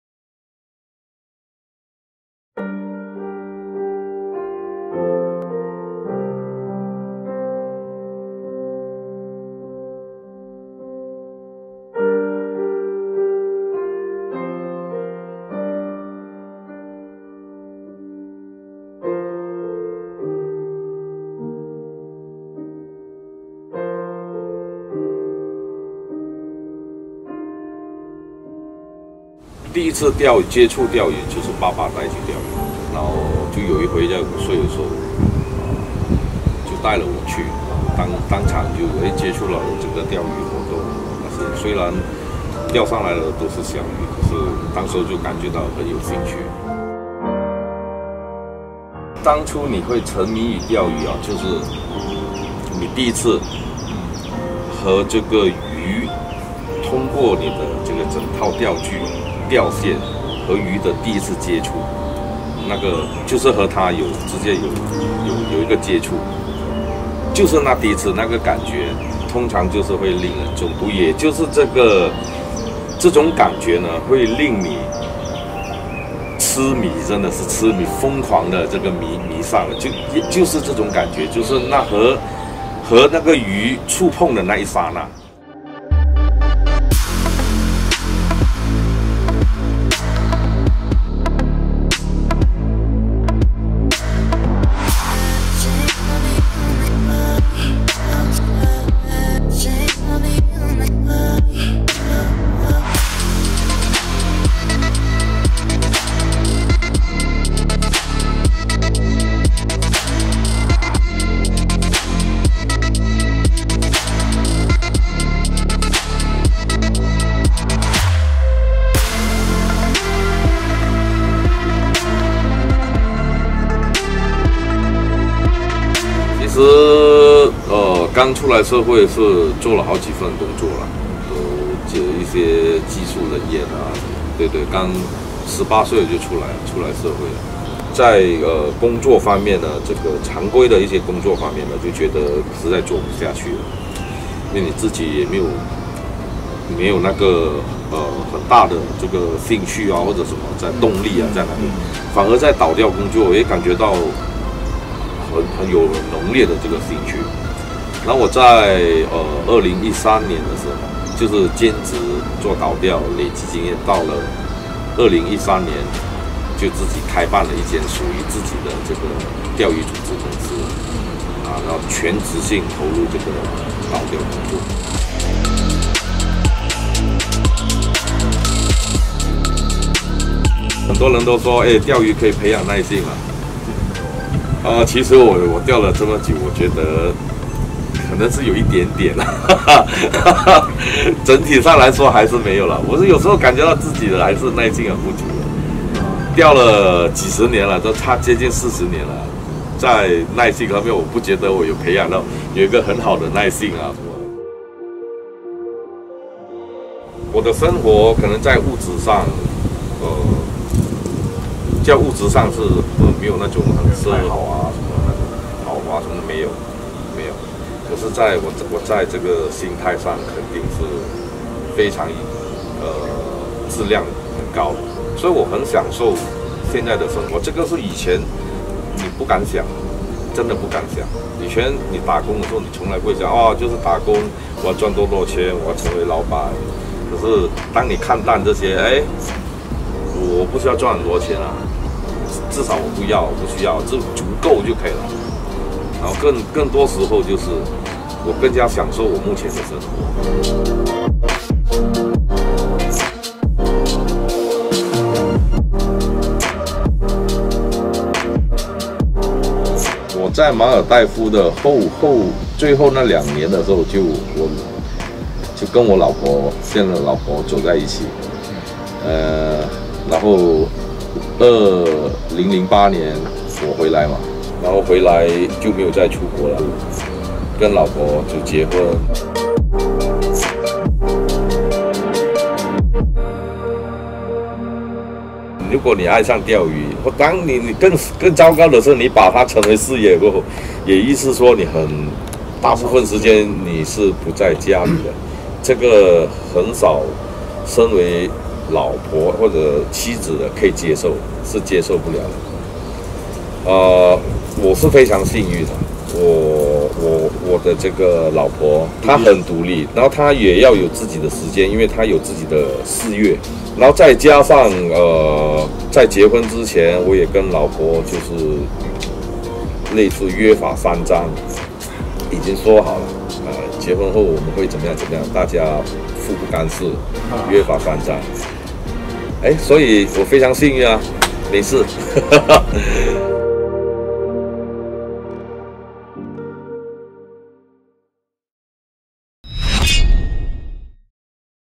BKK - Fishing in West Papua with Jacky Chang
We reached a remote area in West Papua to chase the solid barramundi and papuan black bass roaming in those waters. The journey was very challenging and we managed to drop by Kuala Lumpur, Malaysia to pick up our Ambassador Jacky Chang on the way. The occasion called for some serious talks around the very experience of fishing, something definitely worth sharing.